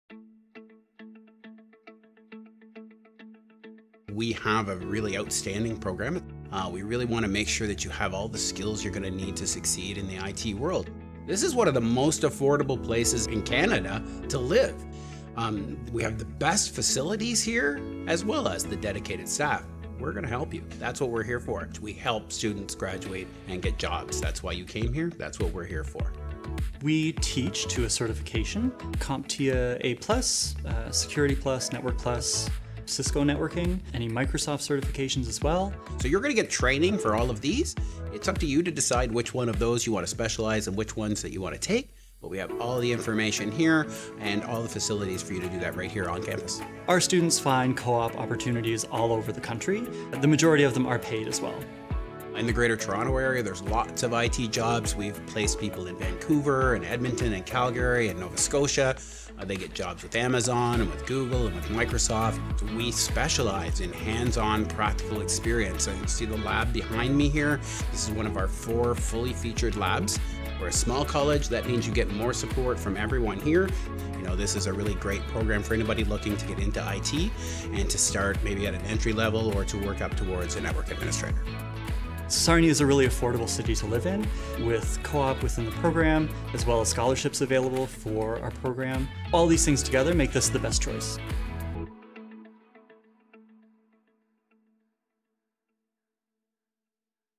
Cloud Infrastructure & Administration | Lambton College
Develop your I.T. skills, graduate, and find a great job in Canada.  Our faculty are here to help!

With a focus on virtualization technologies, server administration, operating systems, network configuration, security, and more, the Cloud Infrastructure & Administration (CIAS) program is a terrific choice!